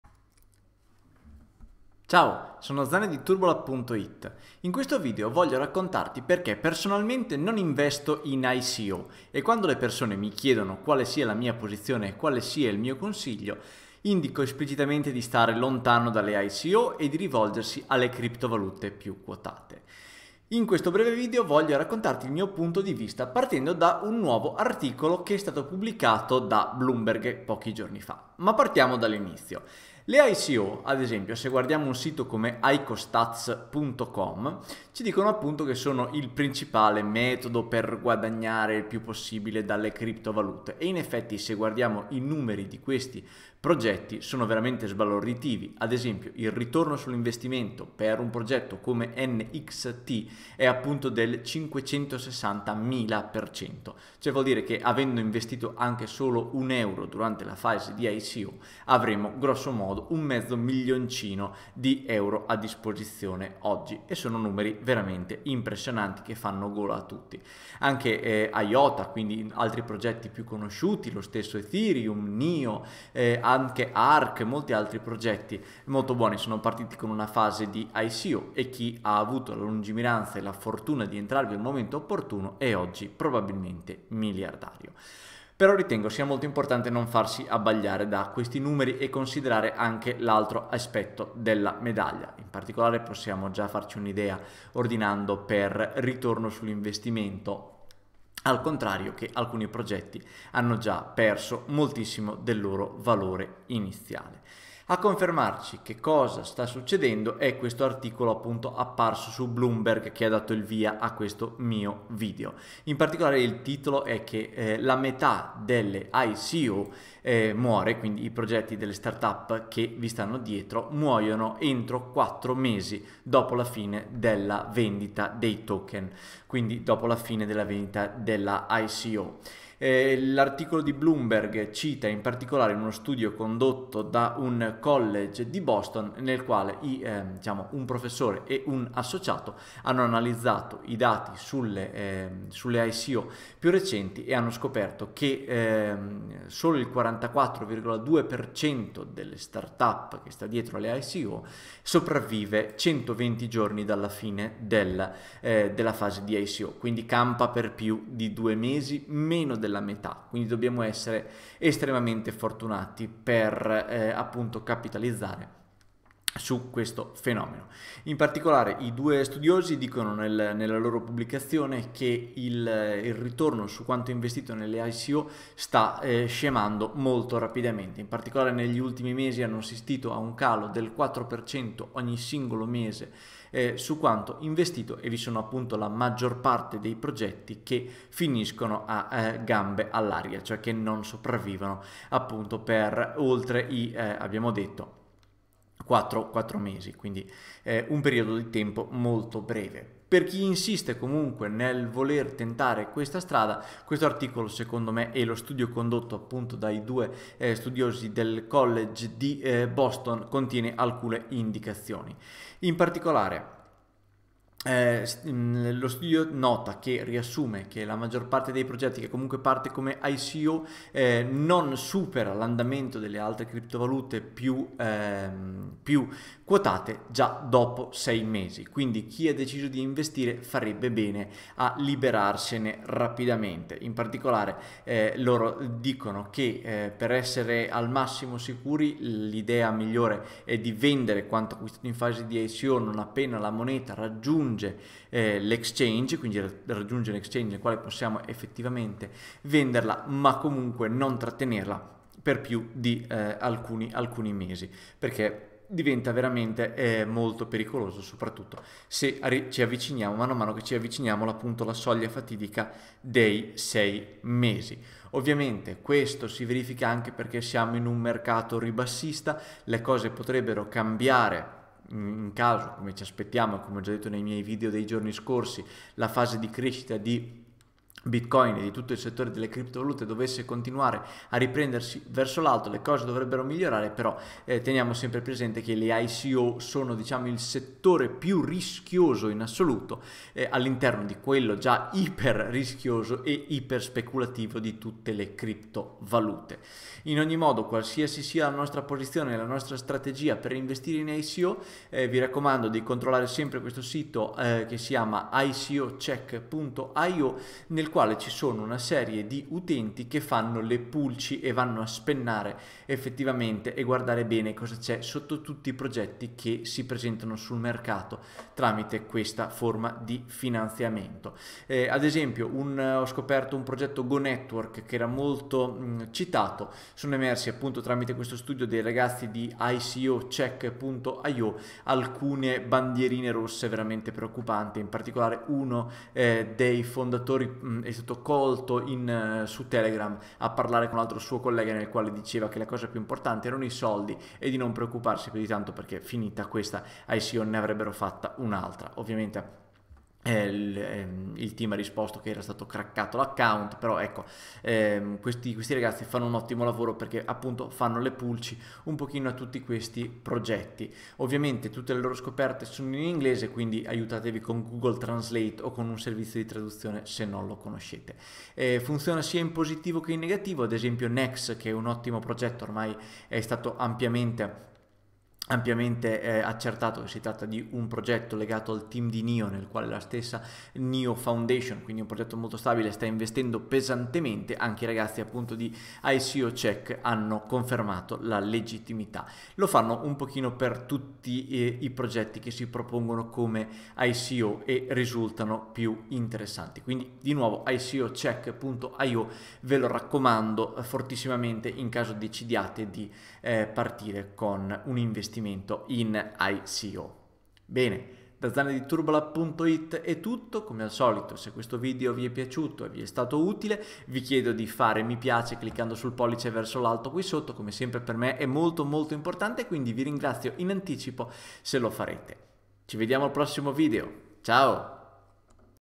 Investire in ICO è pericoloso! I consigli degli esperti per ridurre i rischi
Uno studio rivela che la maggior parte delle ICO fallisce dopo 4 mesi. E i guadagni stanno precipitando. Per non rischiare, meglio vendere subito.

Tu cosa ne pensi? Investi in ICO? Qual è la tua strategia? Scambiamoci le opinioni nei commenti.